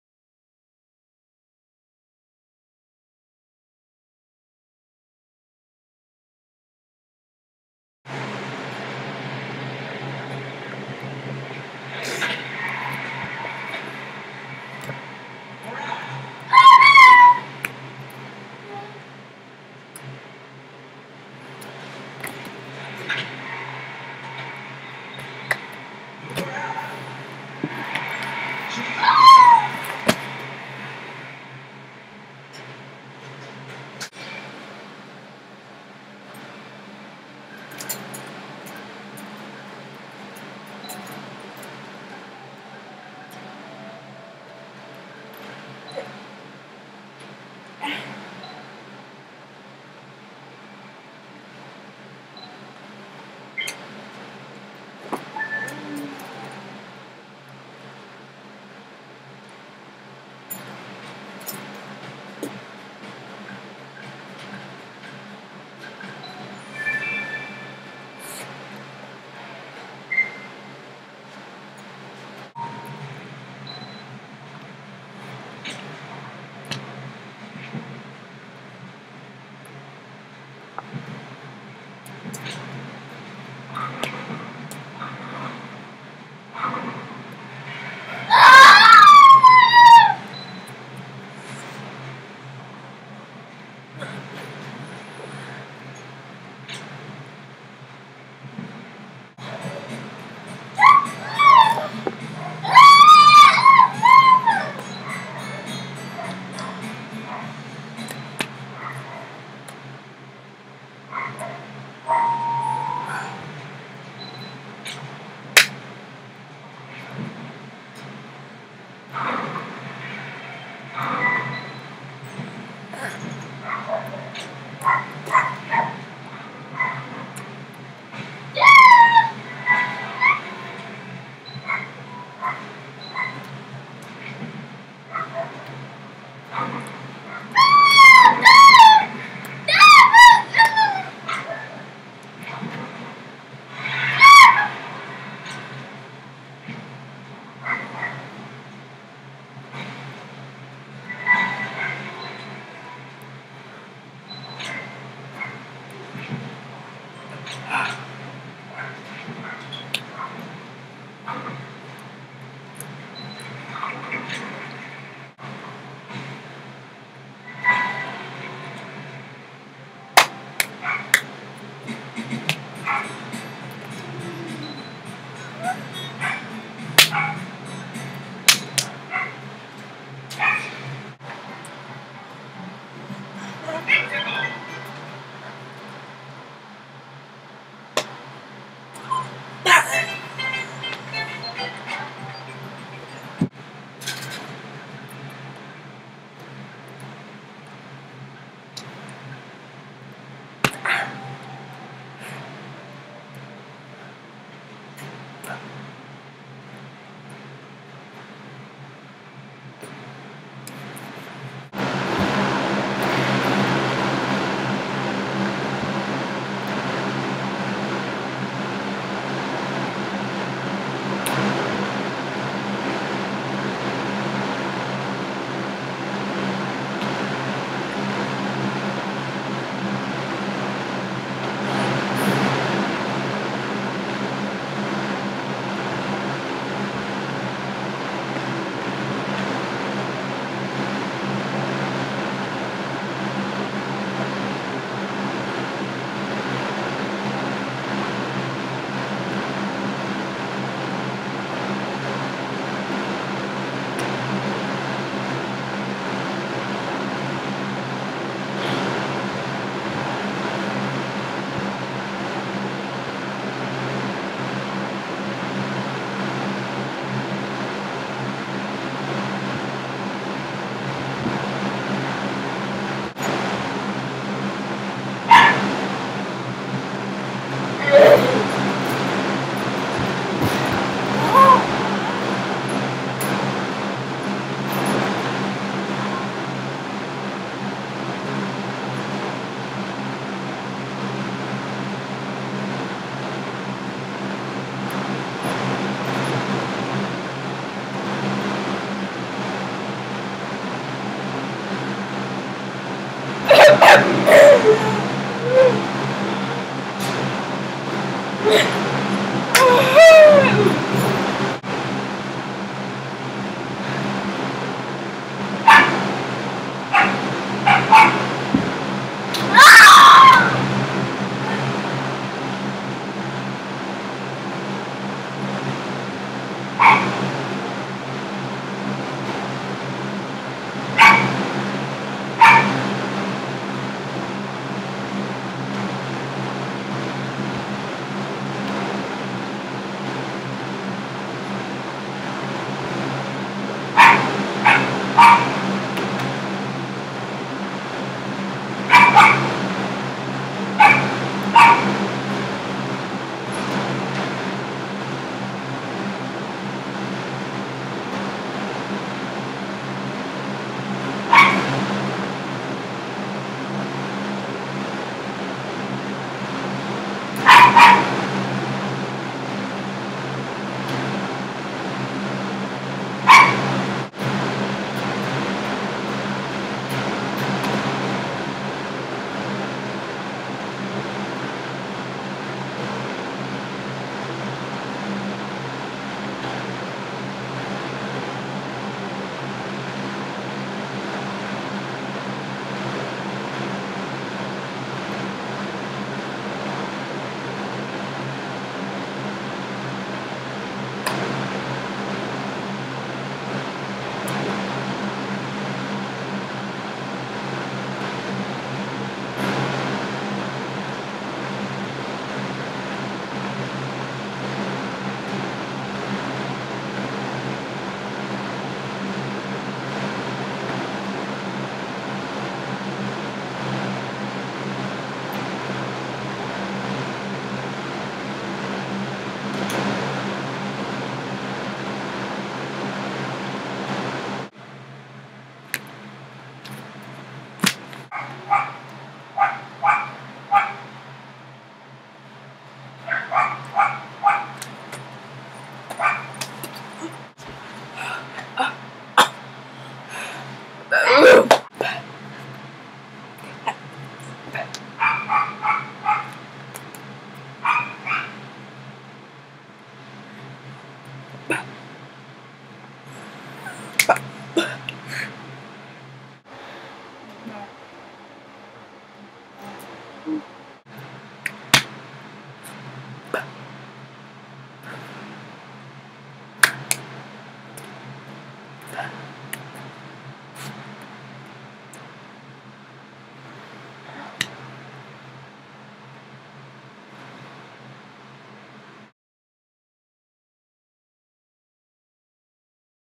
nerd girl VS popluar girl
Young deaf actors played as nerd girl and popular girl. Please rate this and like it too! Please enjoy this story. The story talked about nerd girl wish that she become popular, will she?